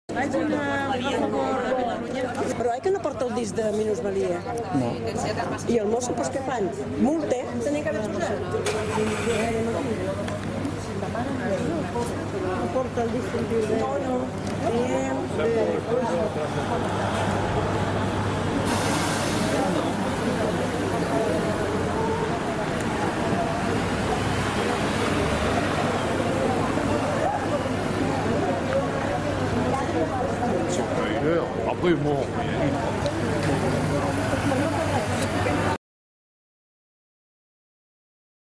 Grava Demòcrates i Sense Acreditació invàlid aparca en zona. Multarà la GU Tarragona?
10 d'octubre de 2019, a les 13 hores, als Jutjats de Tarragona.